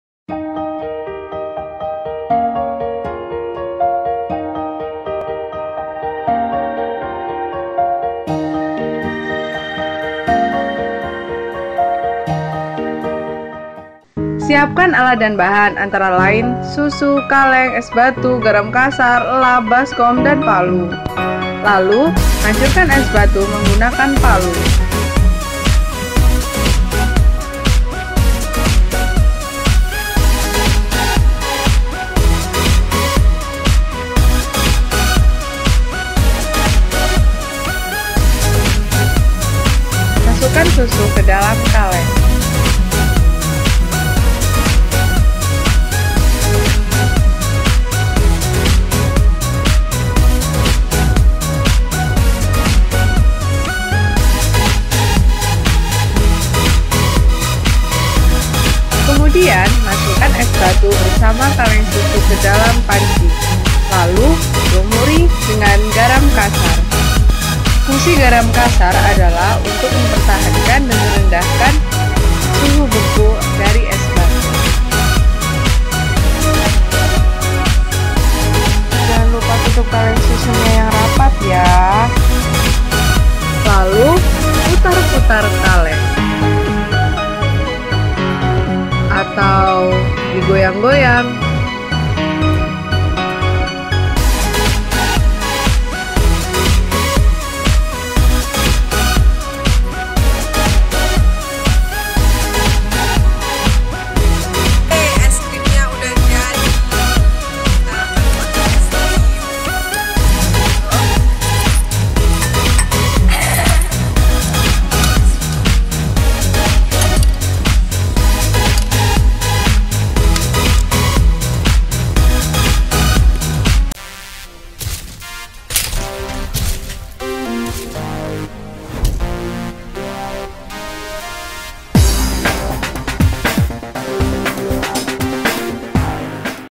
Cara Membuat es Krim yang Sederhana dan Lezat
Beginilah resep dan cara membuat es Krim yang mudah dan sederhana.